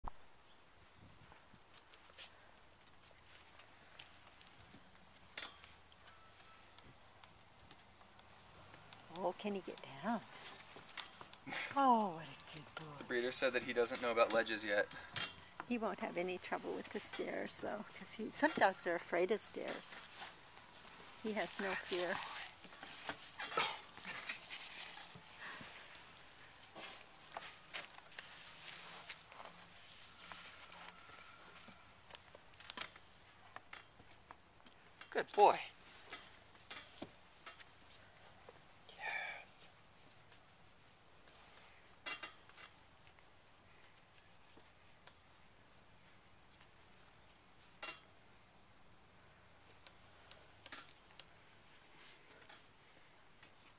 Littlefoot with DAVID
Littlefoot plays with DAVID in his first hour at home